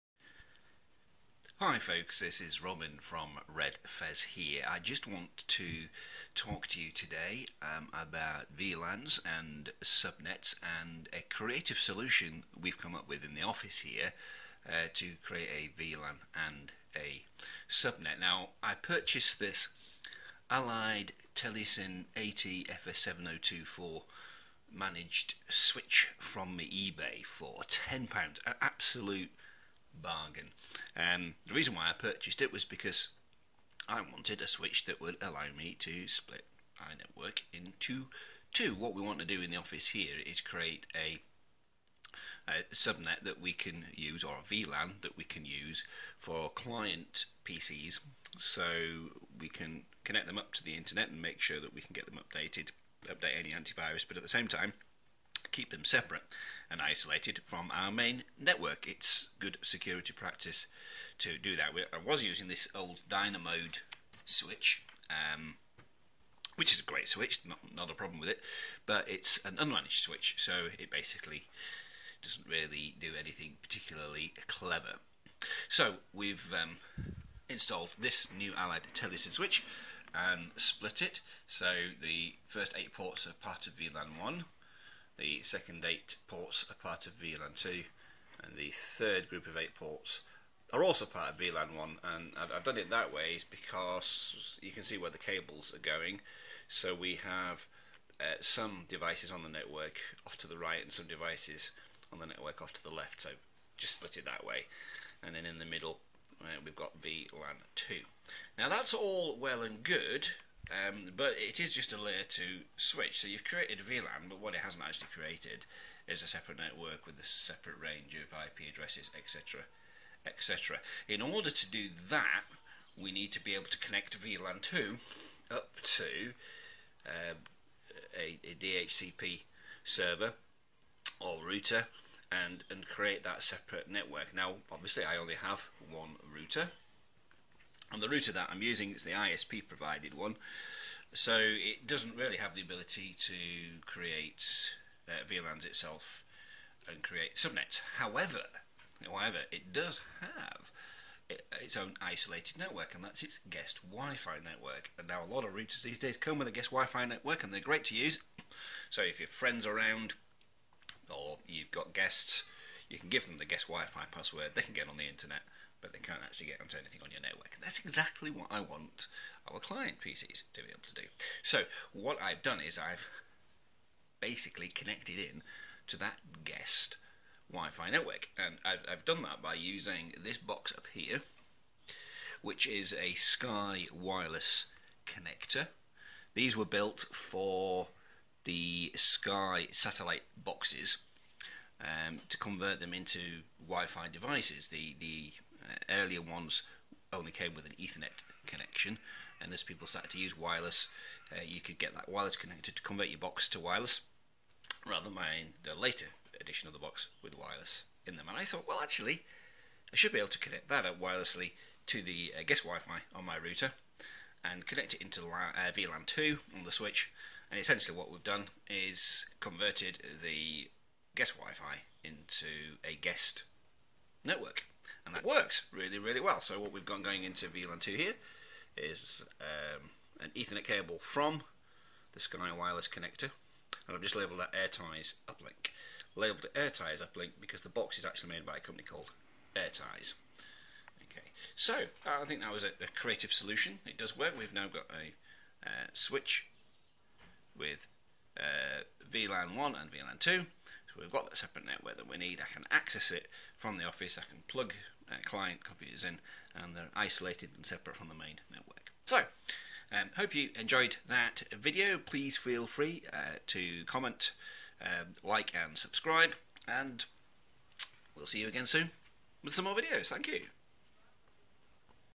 Setting Up a VLAN and subnet using an Allied Telesyn switch and guest wifi
In this video I use a cheap, secondhand managed switch and an old Sky Wireless Connector to turn my guest wifi into a separate, wired network